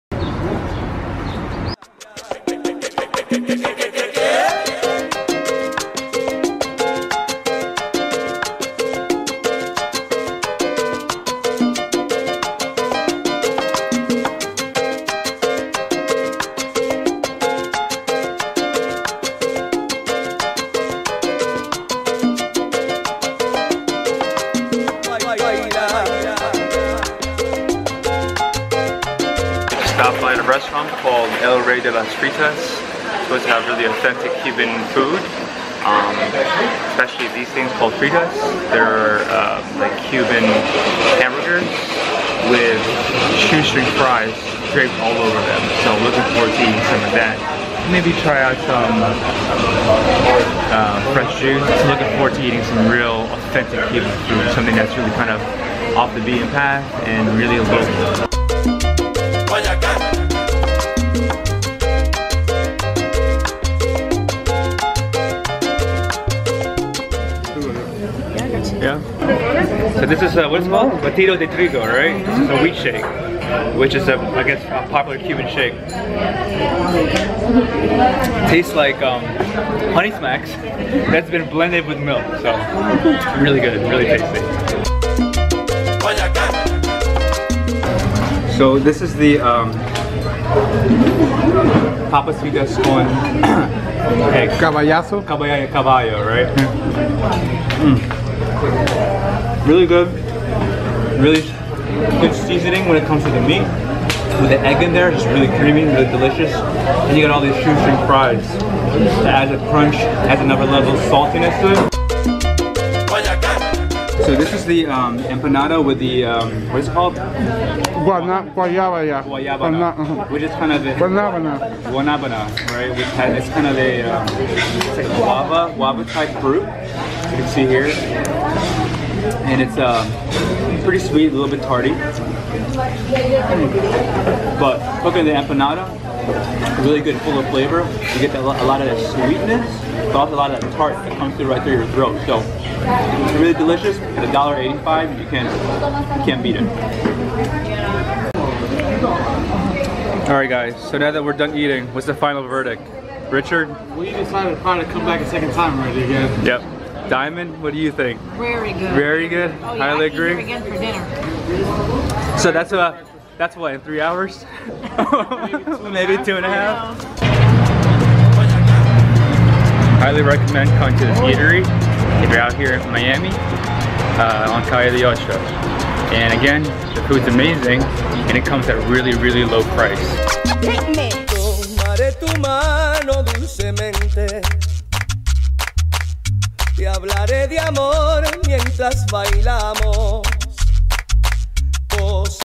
El Rey De Las Fritas Review | MUST EAT FOOD IN MIAMI | Amazing Authentic Cuban Food | MRE 01
MRE 01.  Must eat food in Miami! Little Havana embodies all that Cuban culture offers, including their cuisine! Right on Calle de Ocho, a little eatery called El Rey de las Fritas offers up some of the tastiest authentic Cuban food as incredibly budget-friendly prices!

What I Samples:
➢ Papas Fritas Con Caballo - Cuban Burger with Egg
➢ Batidos de Trigo - Puffed Wheat Milkshake
➢ Guanabana Empanada - Sweet Pastry Filled with Soursop Fruit